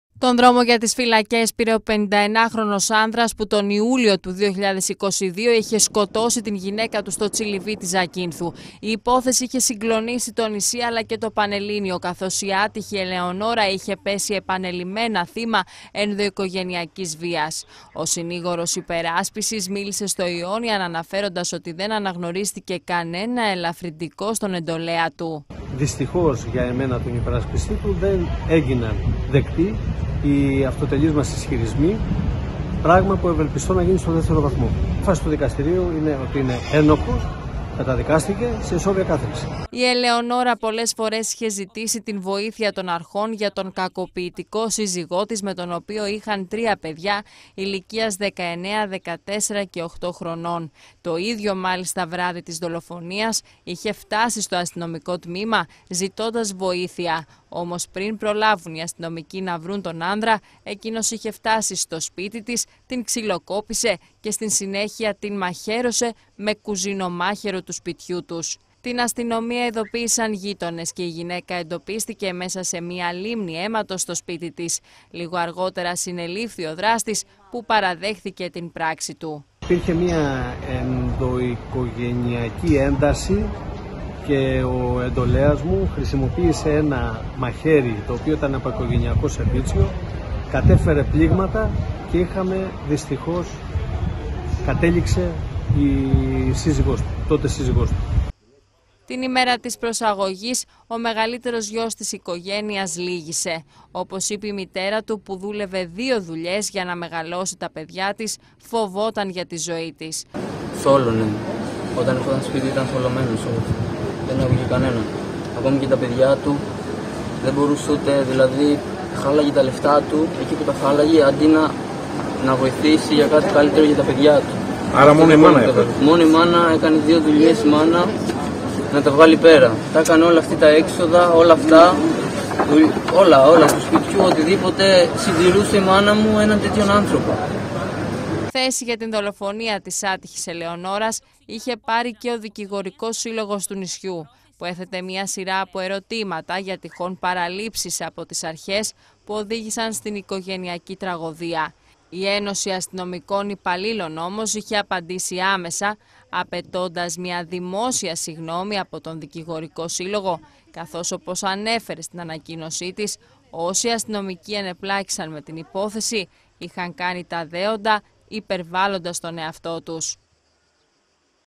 Ισόβια στον συζυγοκτόνο - Το χρονικό της οικογενειακής τραγωδίας
Σε ισόβια κάθειρξη καταδικάστηκε ο 51χρονος που τον Ιούλιο του 2022 μαχαίρωσε και σκότωσε την γυναίκα του στην Ζάκυνθο. Η γυναικοκτονία είχε συγκλονίσει το πανελλήνιο καθώς η άτυχη γυναίκα είχε ζητήσει επανελλειμένα βοήθεια απο τις αρχές για την κακοποιητική συμπεριφορά του συζύγου της.